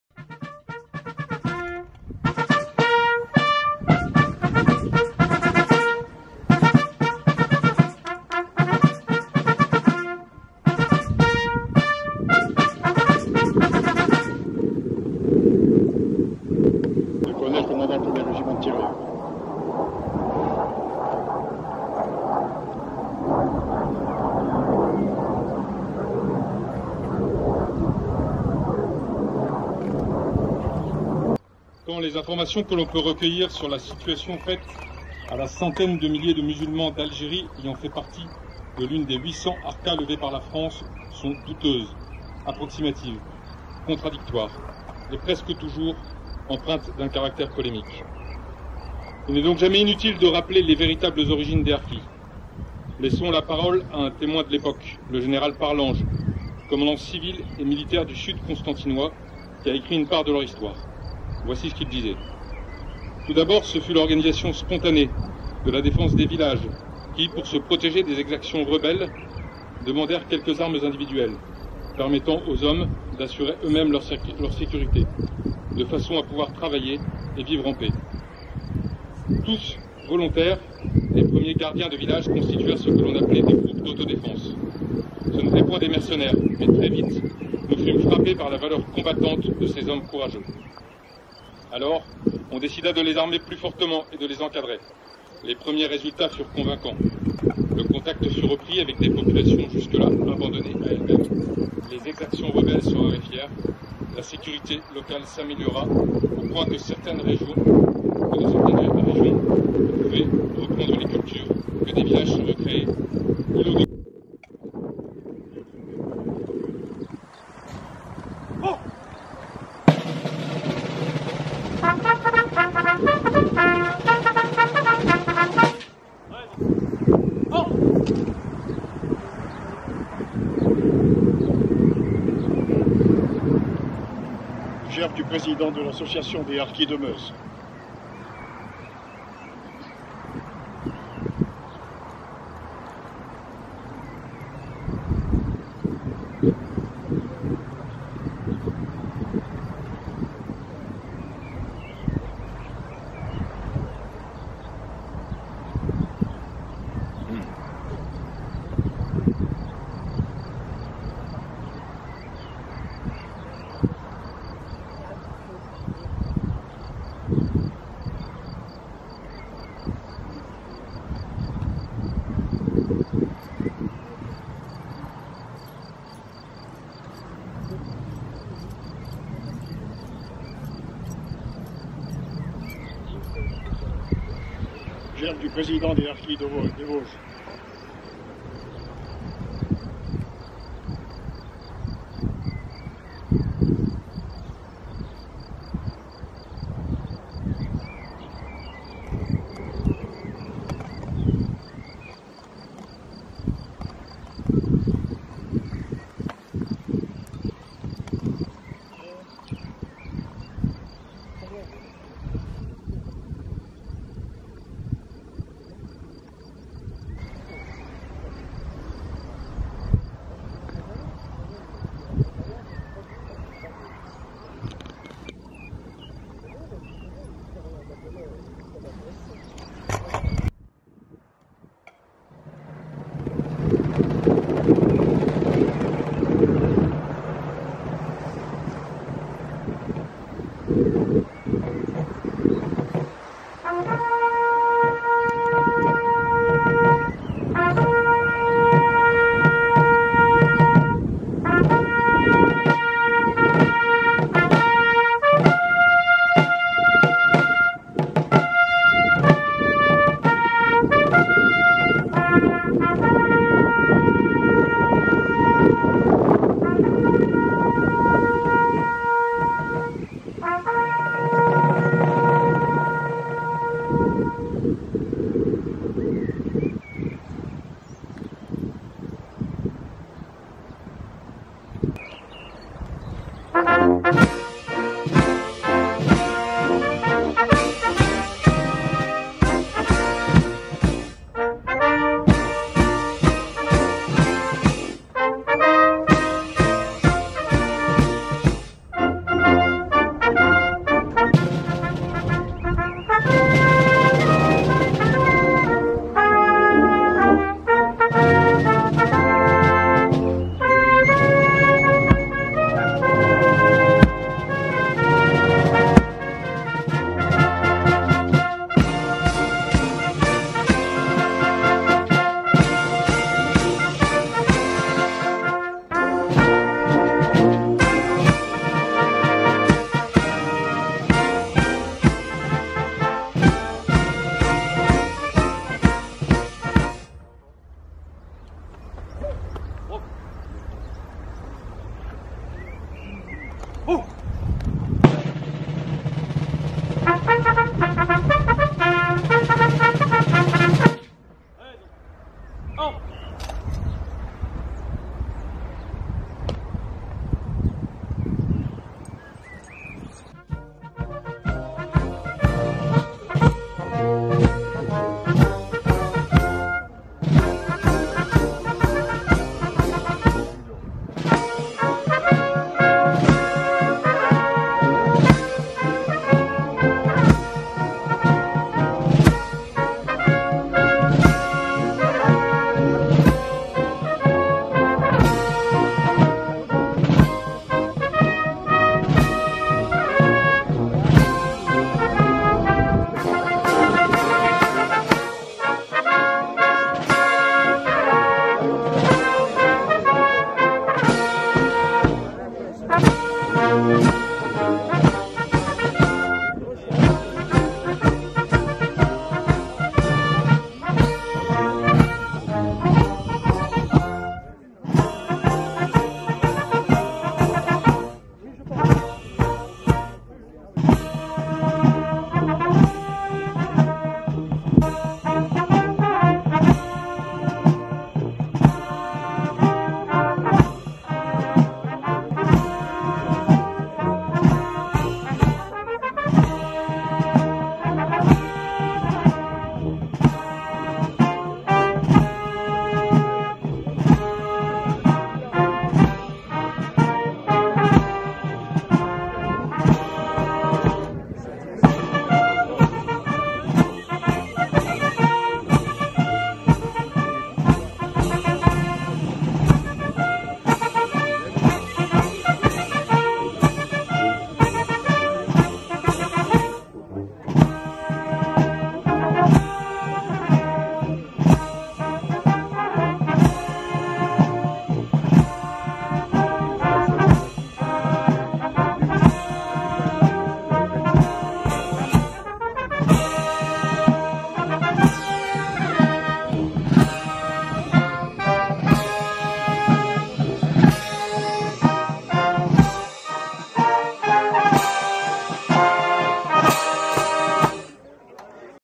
inauguration harki 1
Inauguration d'une stèle en reconnaissance aux Harkis et aux membres des formations supplétives morts pour la France en Algérie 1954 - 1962. avec le concours du 1er régiment de Tirailleurs d'Epinal.